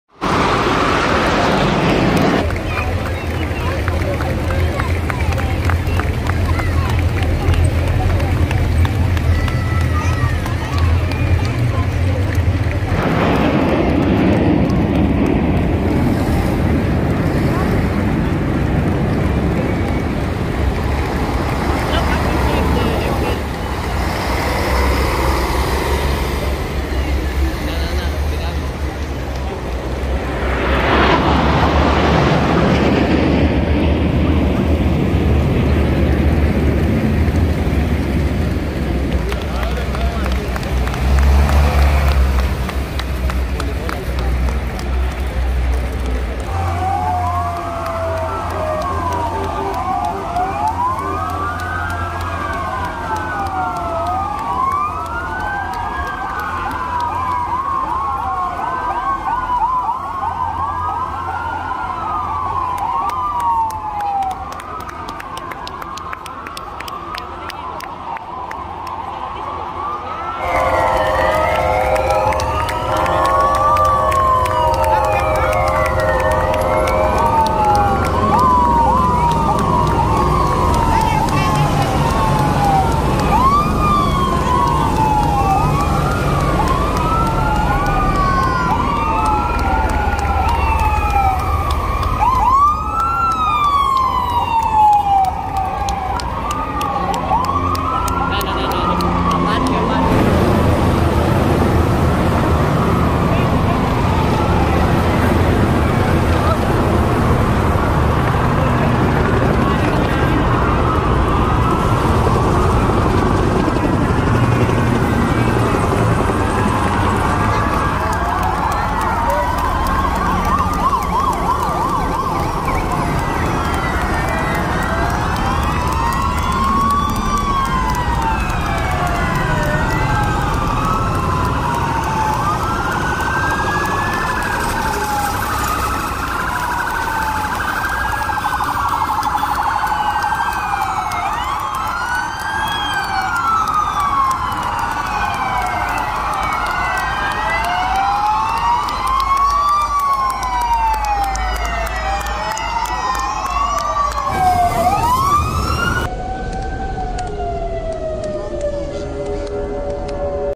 Greek independence day 2023#Military weapons and airplane exhibition
Military different heavy weapons parade independenceday 2023